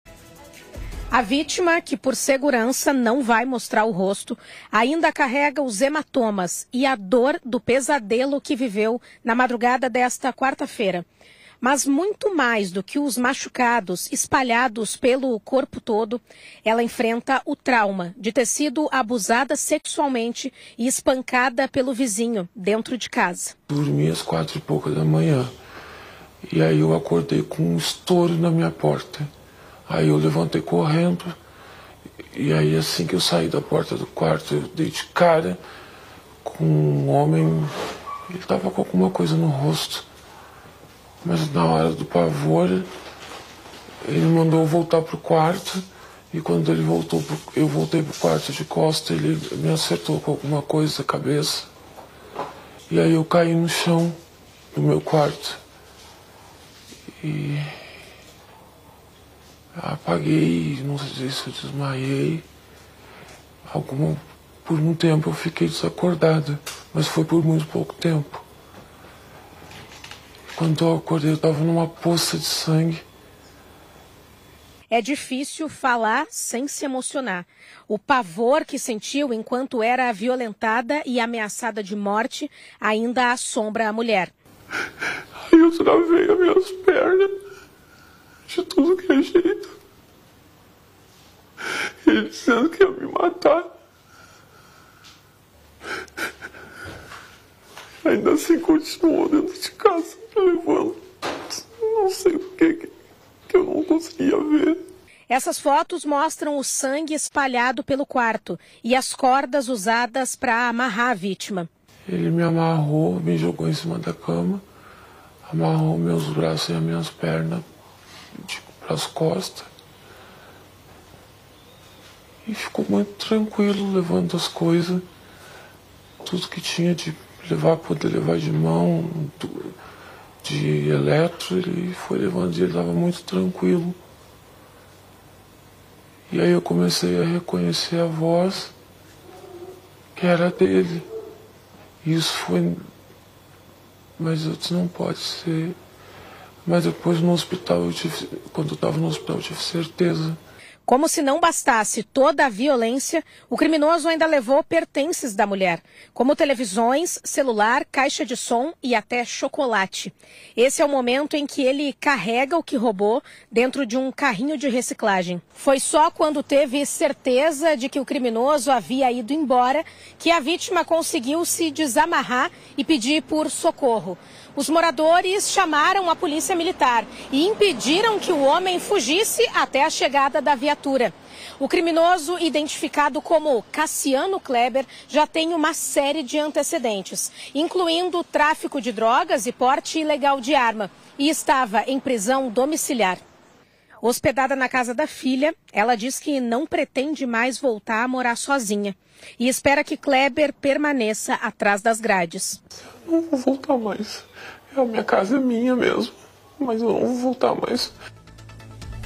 Canoas: idosa é estuprada e agredida dentro de casa
Uma idosa foi estuprada e agredida dentro de casa, em Canoas, região metropolitana. O homem, que é vizinho dela, ainda roubou alguns pertences e fugiu. Mas foi linchado por moradores da região e preso em flagrante pela Brigada Militar. A nossa equipe conversou com a vítima.